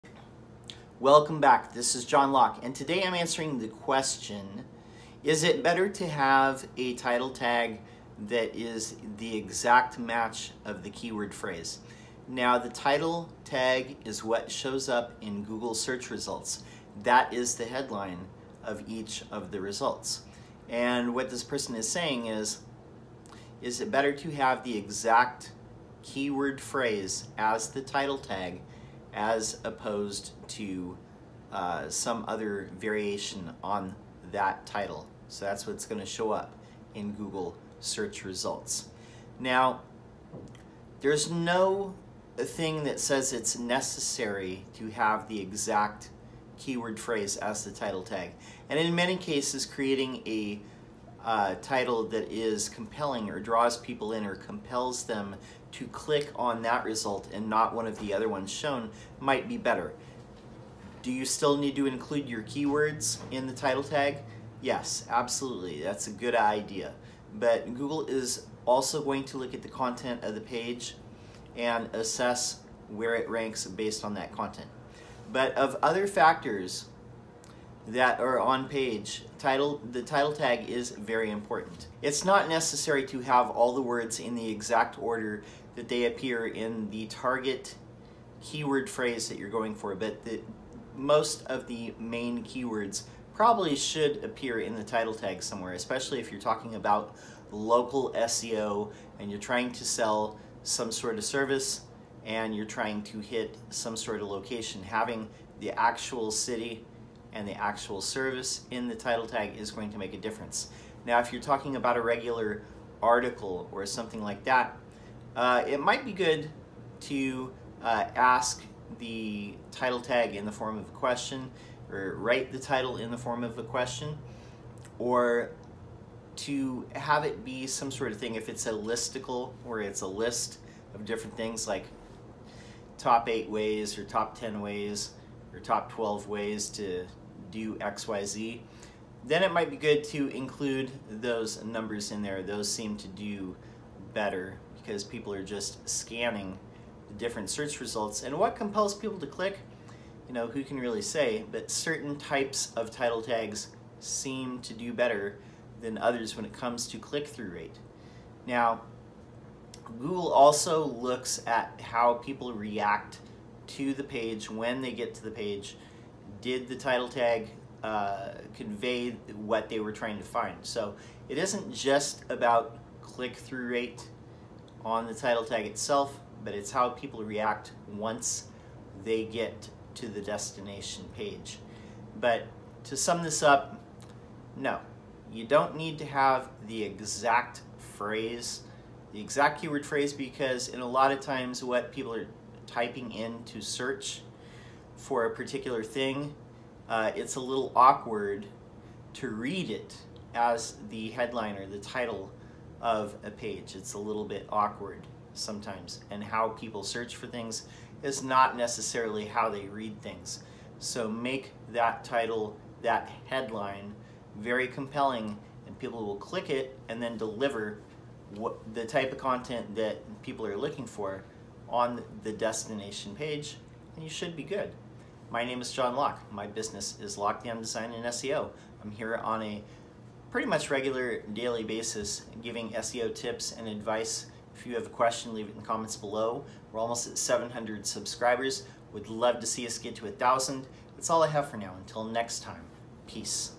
Do Title Tags Need to Match Keyword Phrase Exactly?
Do exact match title tags do better in search rankings? Is it better to have the exact keyword phrase as your title tag? The title tag is what shows up in Google search results.